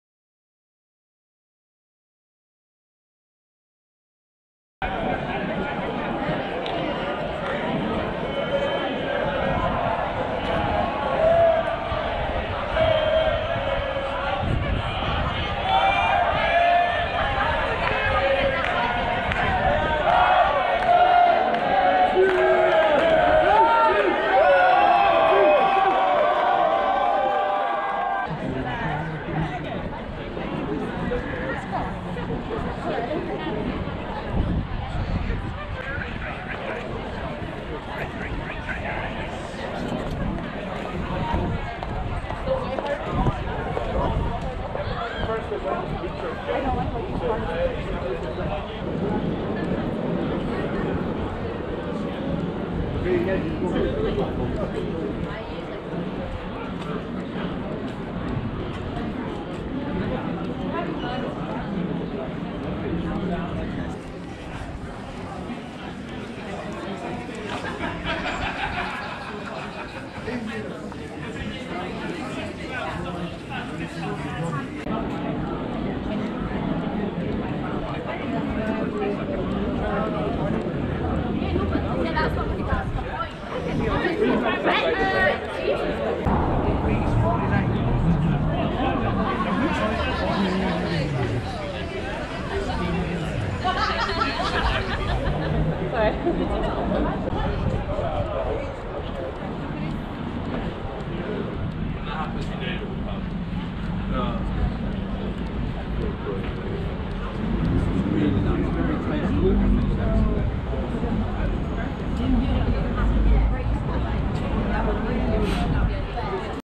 Hospitality Reopens, Monday 26th April 2021
As Scotland moved into Lockdown Level 3 hospitality reopened with alcohol served outside. Having been warm and sunny for days it's now grey, cold and wet but that didn't deter drinkers returning to the Grassmarket, Rose and George Streets.